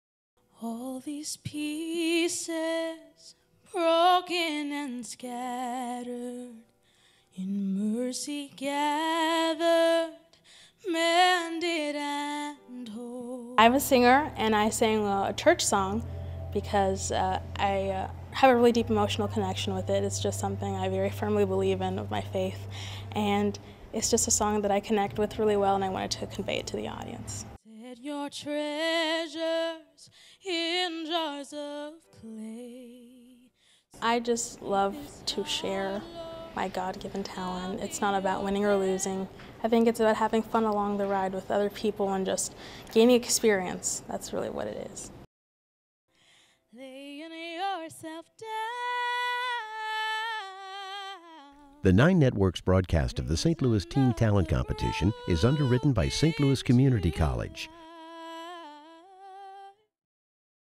Teen Talent Competition 2015 on Nine PBS | Our Story: Audrianna
In the spirit of fostering the emerging talent in the St. Louis performing arts community, the Fox Performing Arts Charitable Foundation is proud to underwrite and produce an adjudicated competition showcasing the splendidly talented teens of the St. Louis Metropolitan area.  Students are competing for college scholarships, special awards, prizes and public appearance opportunities.

The competition will be broadcast on Nine PBS on May 23, 2014 at 8pm.